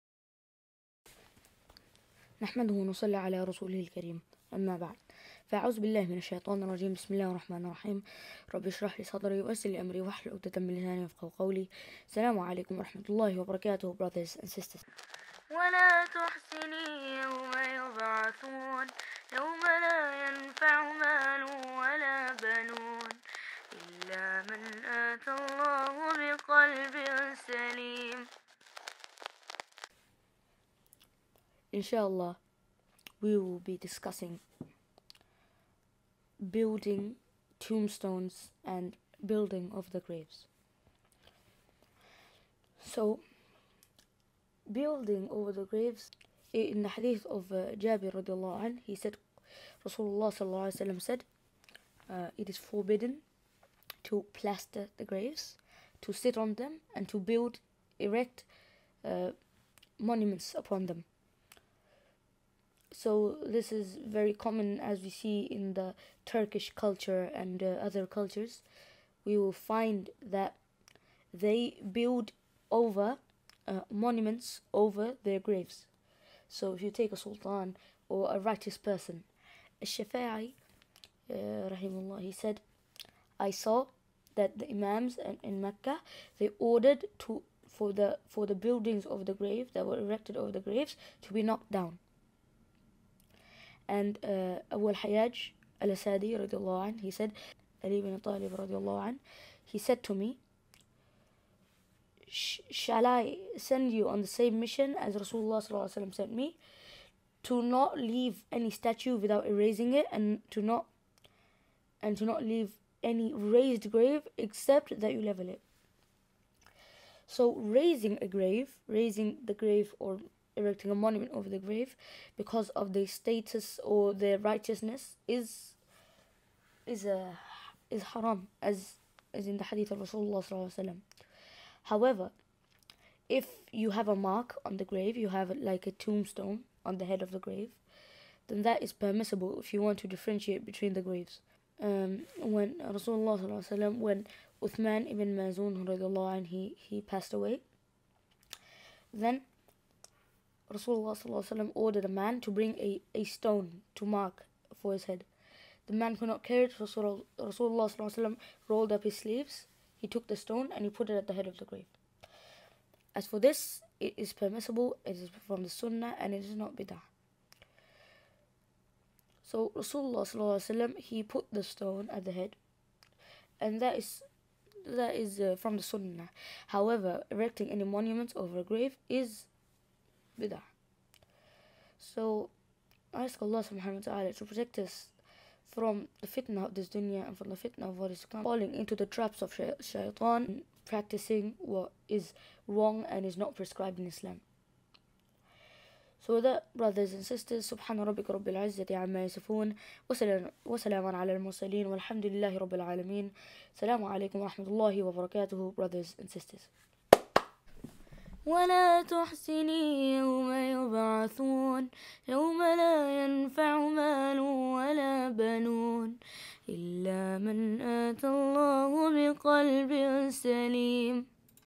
Ep. 3 | Building Over The Graves || Sunnah Or Bid’ah || Br. Sheikh
Have We Been Practicing Bid'ah Unknowingly? What Is And Isn't Bid'ah? How Can We Avoid Bid'ah?, 🧐

It's These Questions That Keep Us On Our Toes, In This Series Br. Sheikh Will Insha Allah Answer All These Questions

JazakAllahu Khairan 🤲🏻